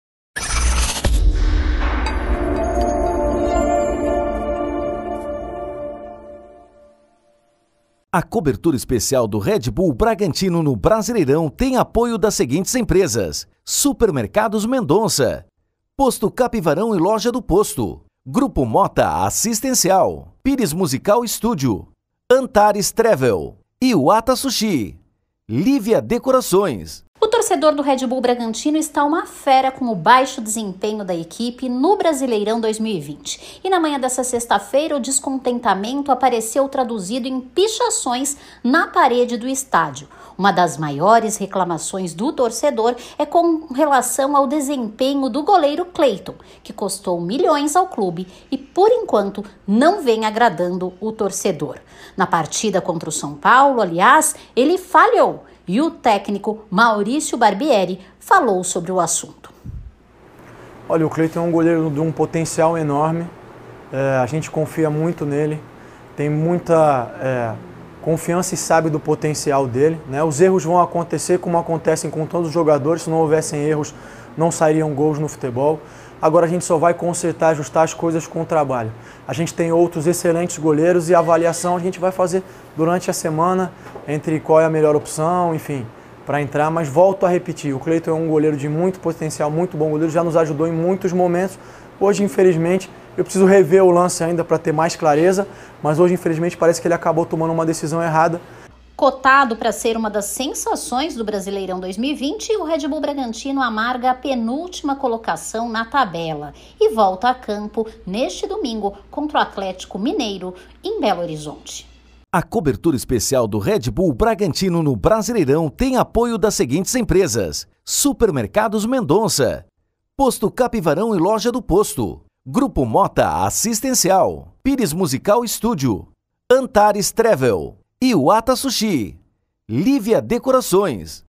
Cadê o futebol? Perguntam torcedores do Bragantino em pichação
Estádio do Bragantino amanhece pichado com cobranças ao goleiro Cleiton. Após falhas será que Barbieri vai mantê-lo no time na partida contra o ex-clube do atleta?
Bragantino enfrenta o Atlético-MG no domingo, 13.